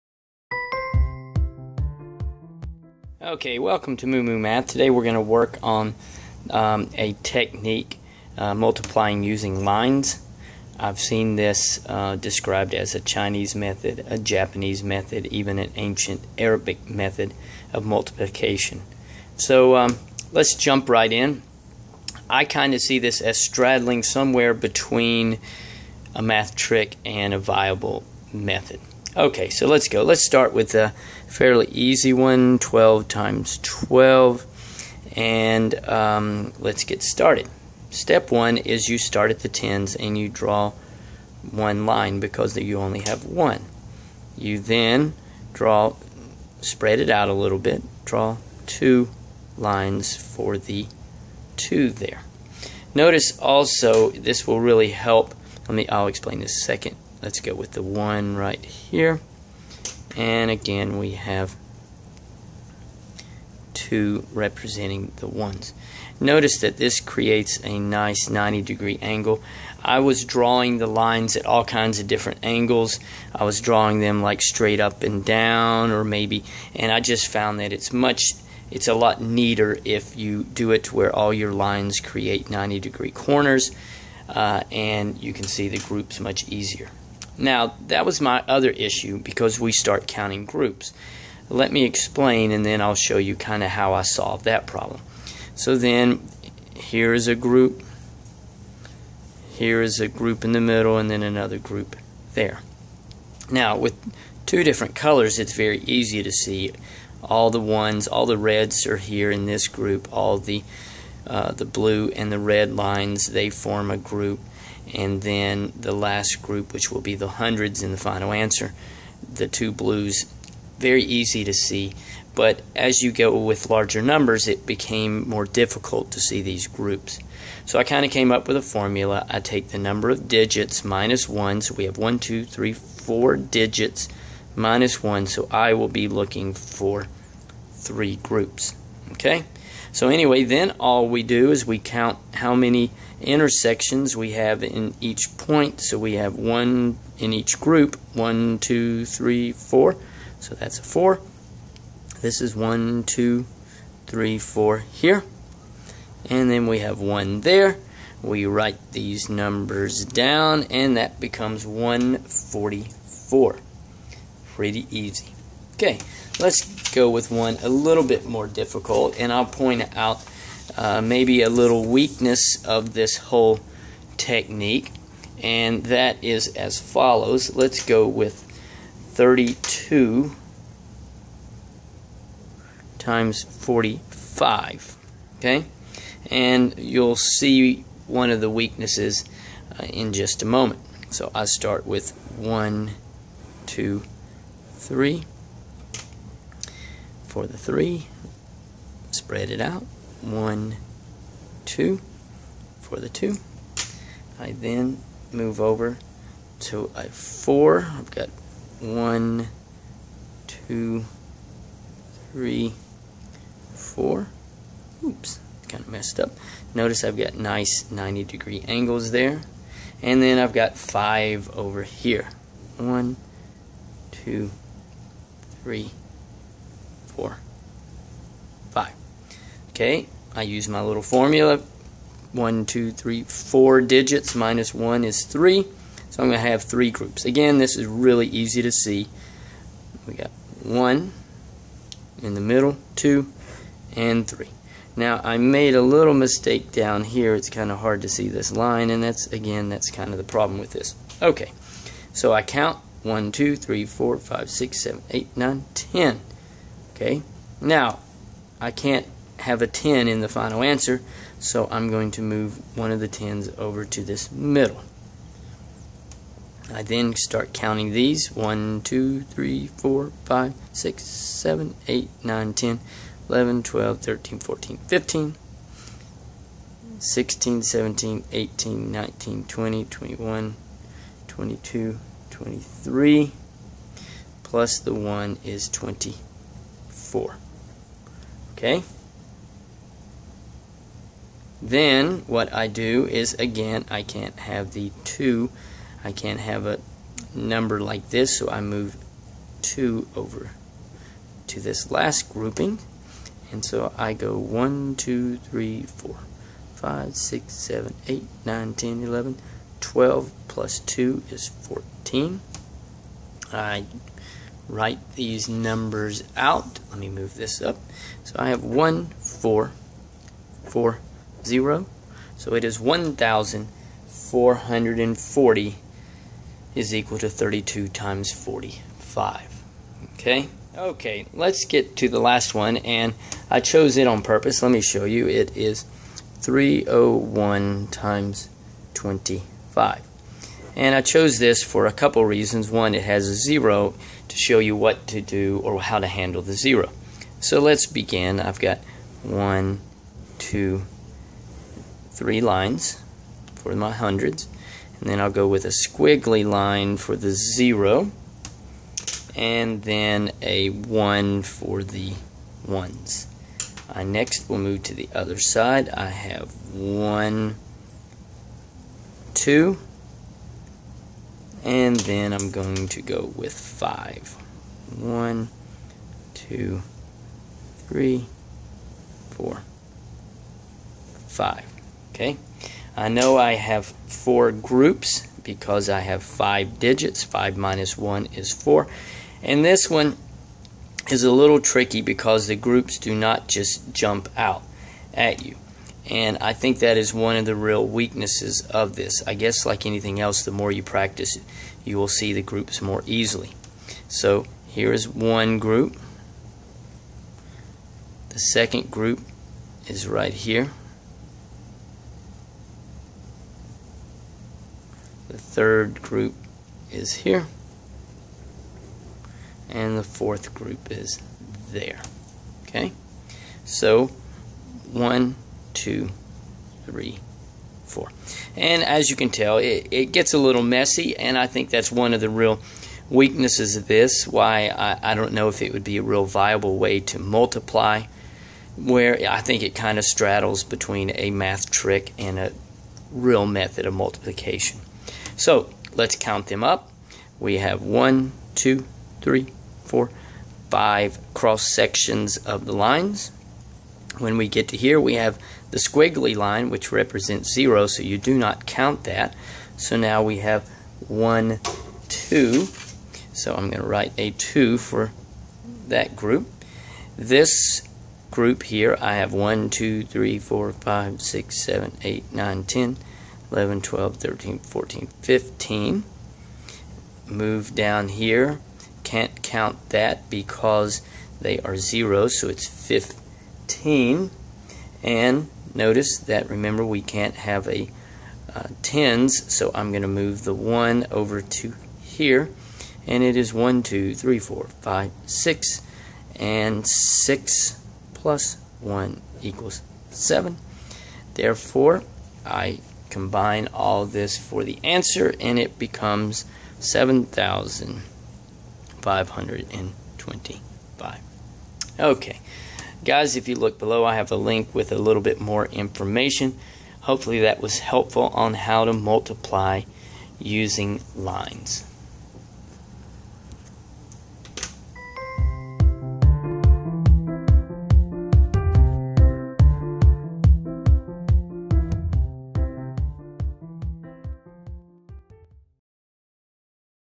Math Tricks-Multiplying with Lines
I have seen this technique described as "Chinese Multiplication", Japanese Multiplication", Vedic Math Multiplication, and even "Ancient Arabic Multiplication". It is a fun way to multiply numbers using lines. I don't know if you can replace traditional forms of multiplication with this technique, but it does work on all numbers.